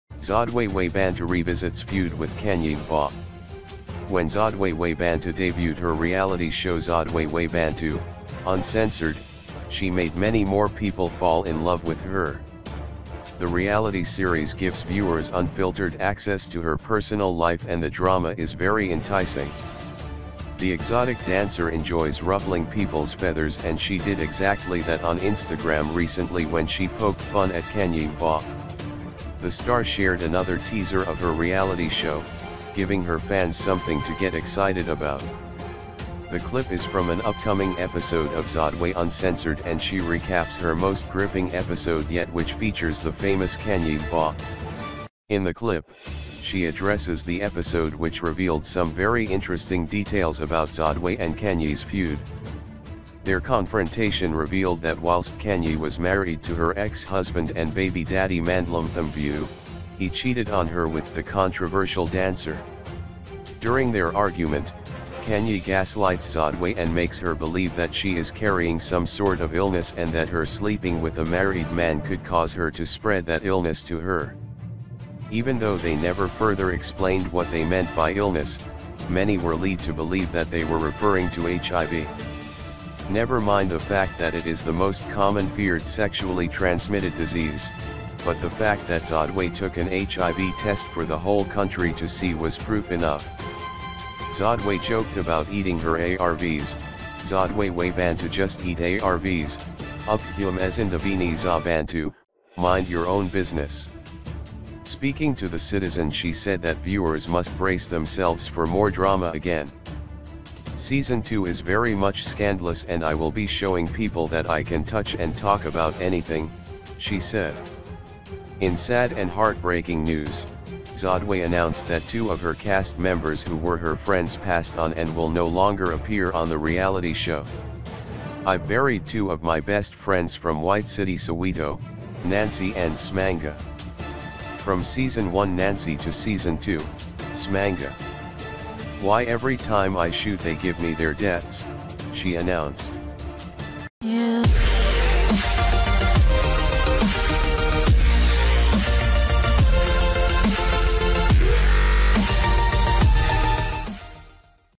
Zodwa Wabantu revisits Feud with Khanyi Mbau
When Zodwa Wabantu debuted her reality show Zodwa Wabantu: Uncensored, she made many more people fall in love with her. The reality series gives viewers unfiltered access to her personal life and the drama is very enticing.